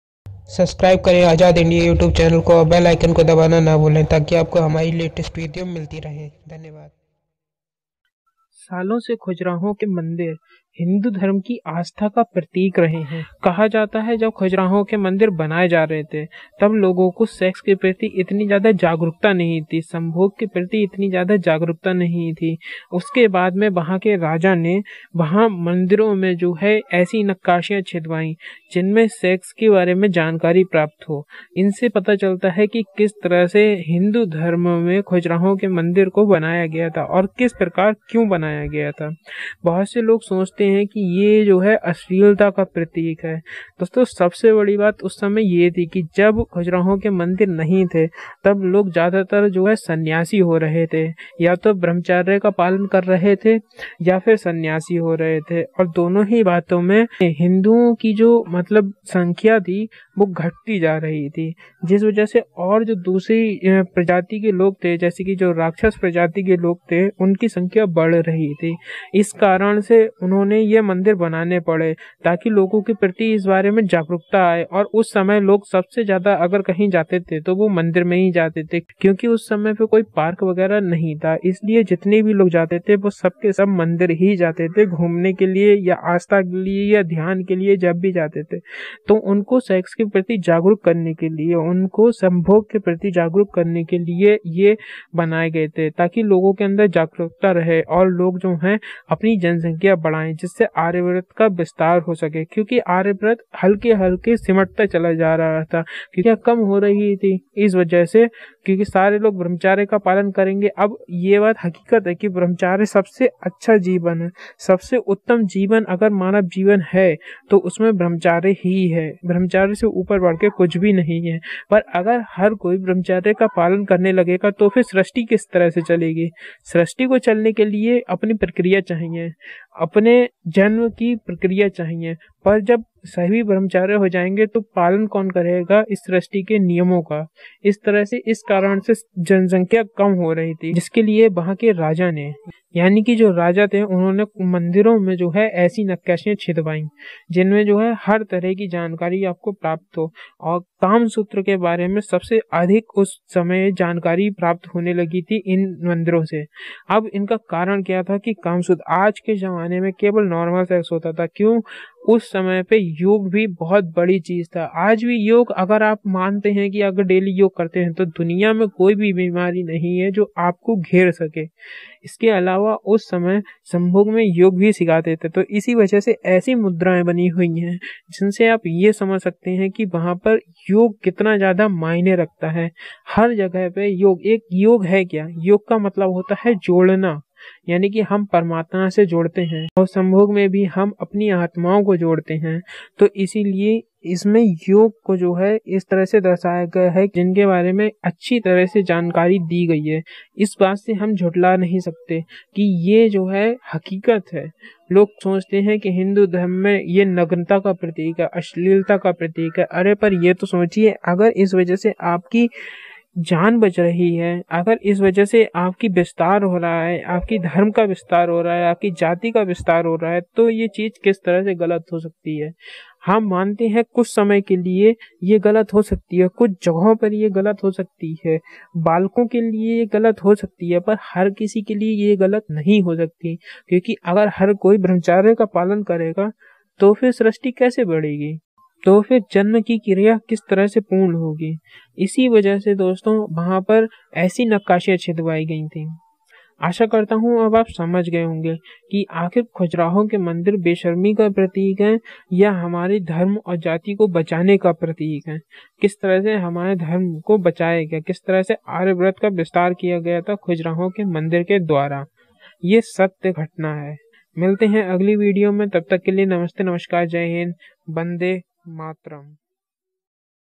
क्या है खुजराहों के मंदिर का रहस्य। भारत के इतिहास में खुजराहों का मंदिर
नमस्कार दोस्तों आज मैं आपको बताऊंगा कि खुजराहों के मंदिर का असली तात्पर्य क्या है
भारत में खुजराहों के मंदिर विश्वप्रसिद्ध है परन्तु उनका मतलब कुछ और ही है